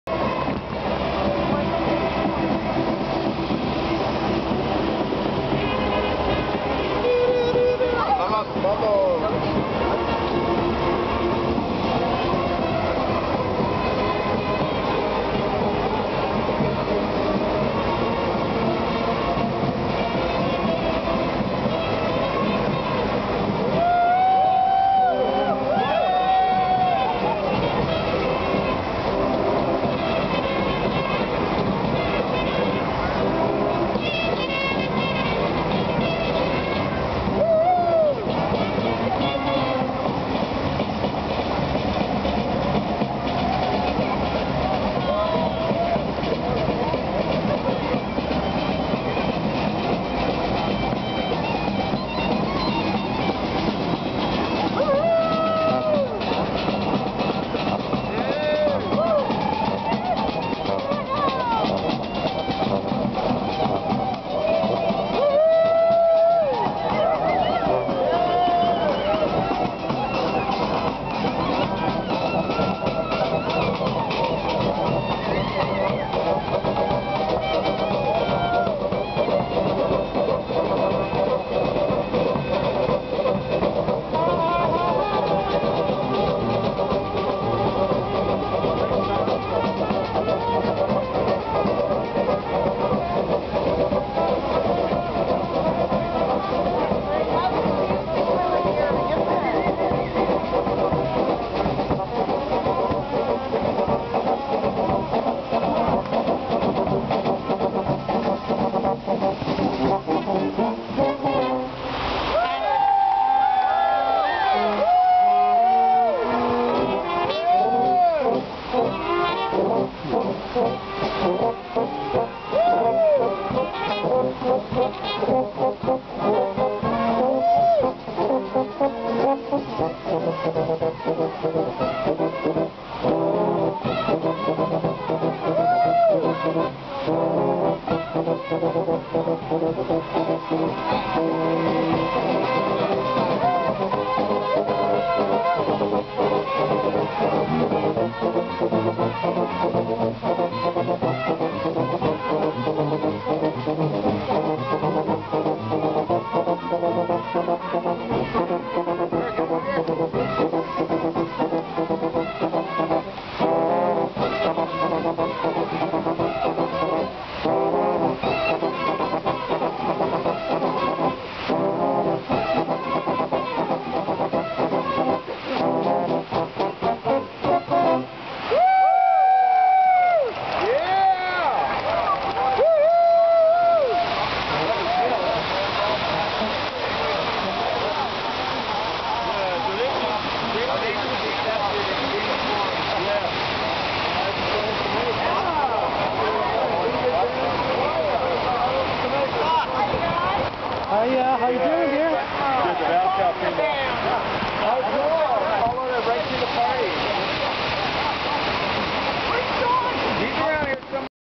VANCOUVER 2011 WORLD MARIJUANA MARCH to END PROHIBITION WEDDING PARTY.AVI
VANCOUVER 2011 WORLD MARIJUANA MARCH to END PROHIBITION A WEDDING APRTY complete with a band!